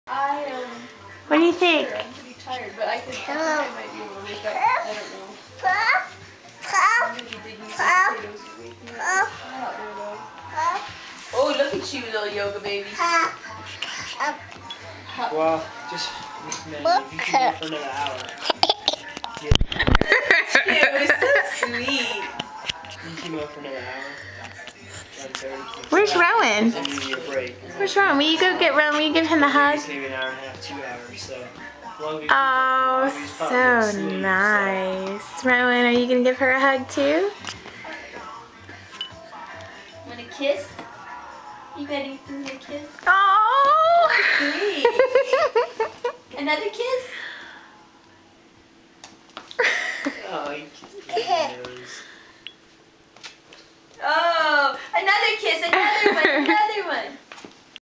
baby kisses
rowan and vivi giving each other hugs and kisses